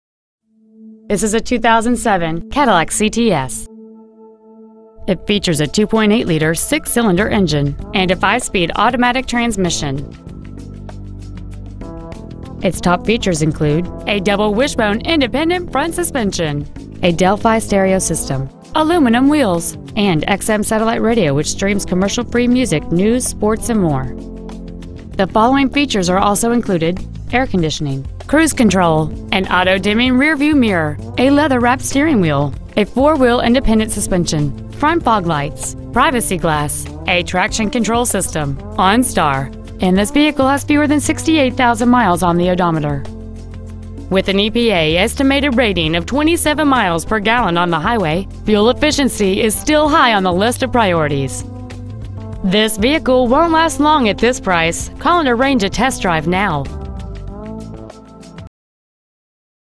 Pre-Owned 2007 CADILLAC CTS Sheffield Village OH
Year : 2007
 Make : CADILLAC
 Model : CTS
 Engine : 2.8L V6 24V MPFI DOHC
 Trans . : 5-SPEED AUTOMATIC WITH OVERDRIVE
 Exterior : LIGHT PLATINUM
 Miles : 67,112
 Interior : EBONY
 Stock : 000E9024